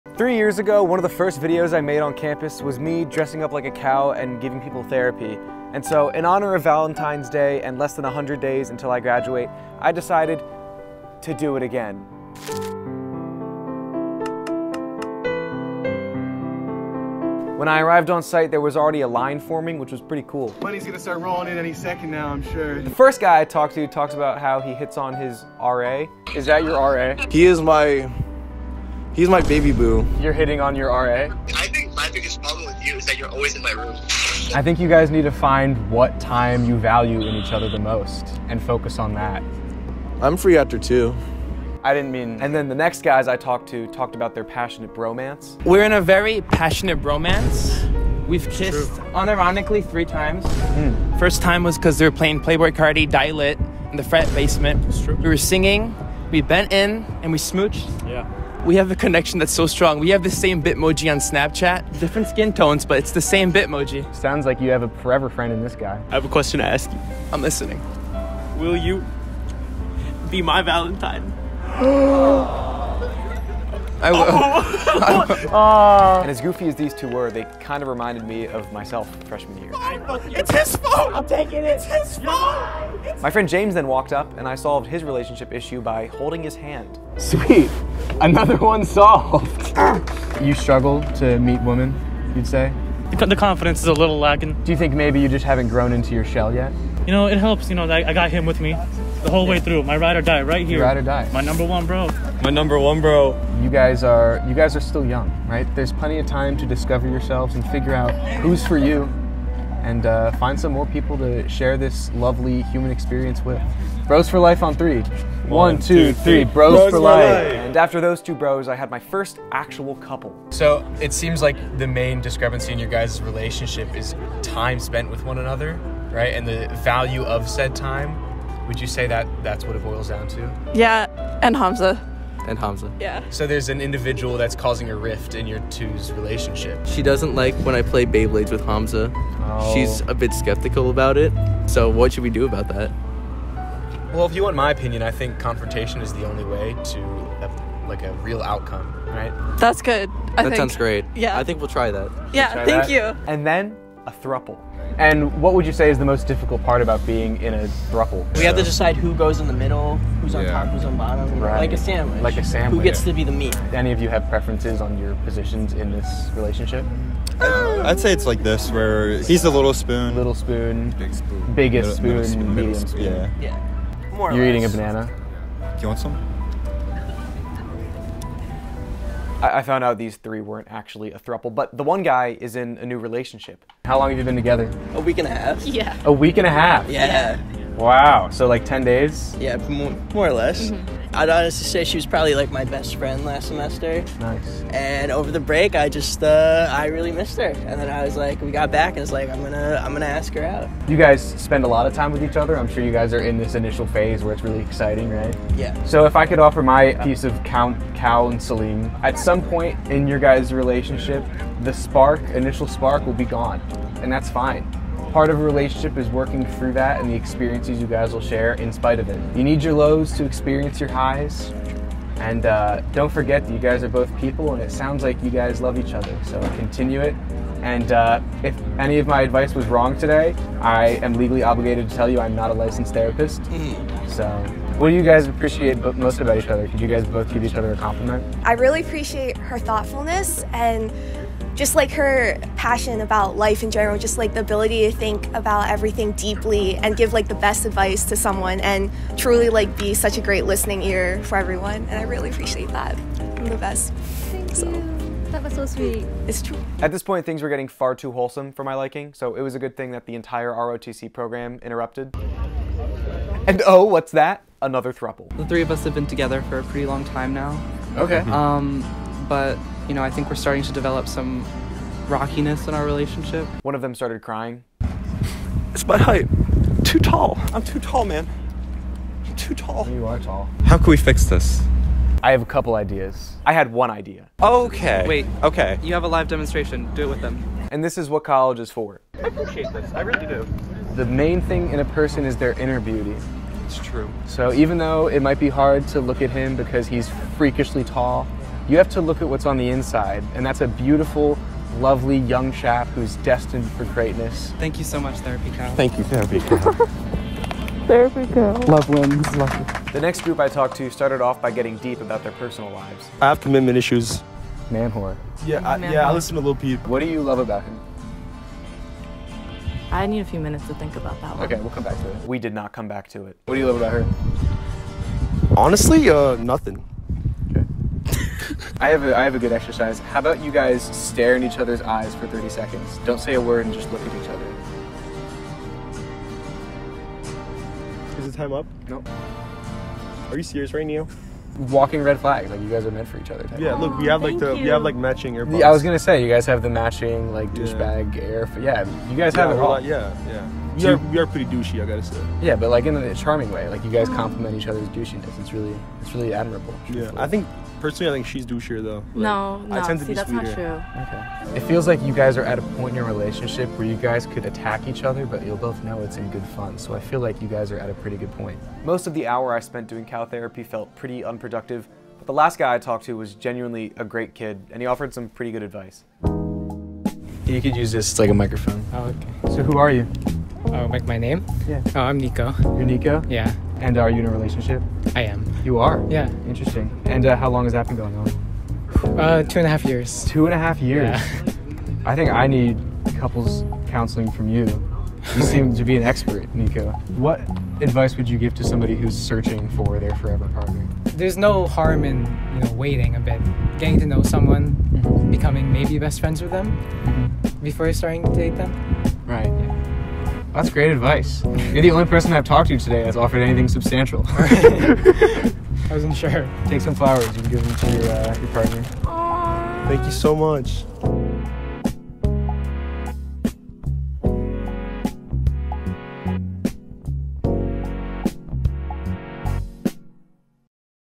couples therapy (but im dressed as a cow)
At my university, I hung posters and did couples therapy as a cow.